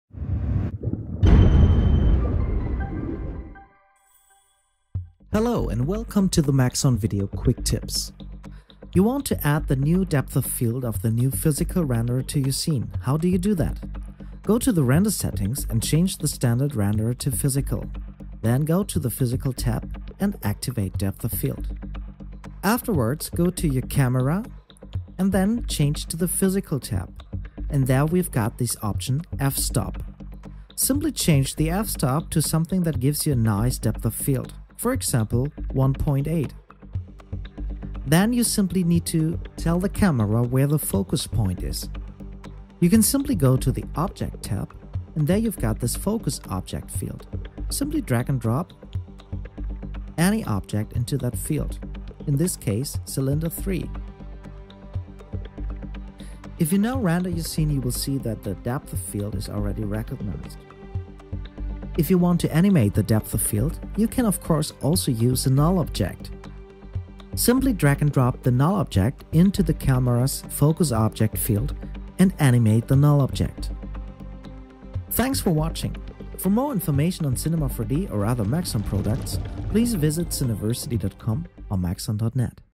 Tip - 92: Realistic Depth of Field in Cinema 4D R13
Find out how to activate and animate realistic depth of field using Cinema 4D Release 13's new physical renderer.